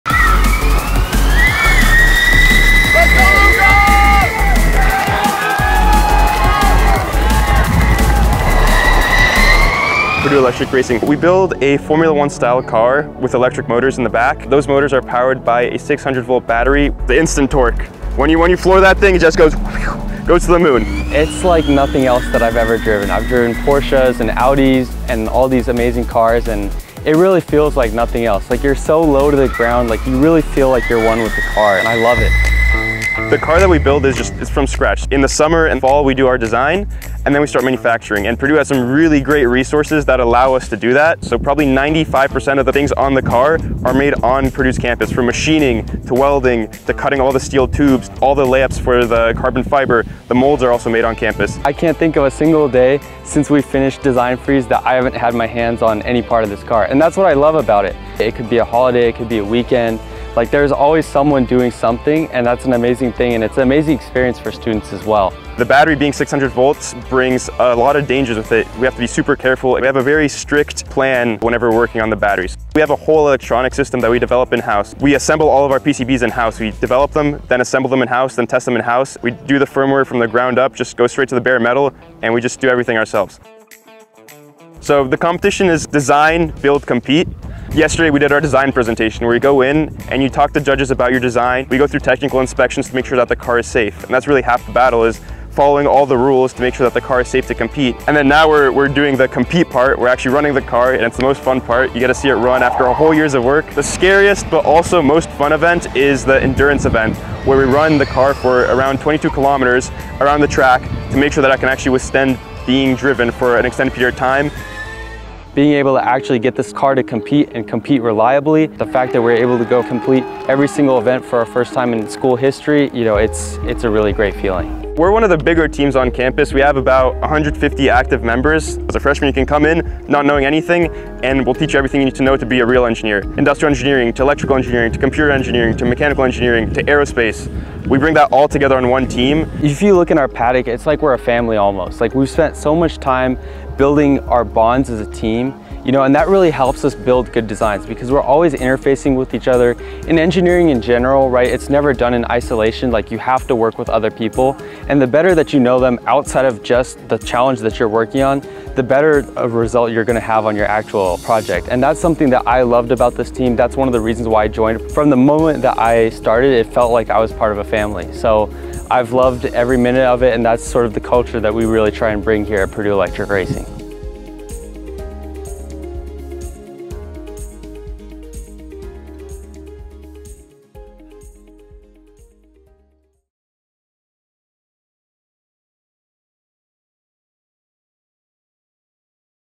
Purdue Electric Racing: Flow With the Current
In most motorsports, the roar of the engine draws people in. But for Purdue Electric Racing, it’s the high-pitched whine of electric motors powered by a 600-volt battery. This year’s Purdue Electric Racing team enjoyed another unique sound — victory. They successfully completed every event in their SAE competition for the first time in their 12-year history.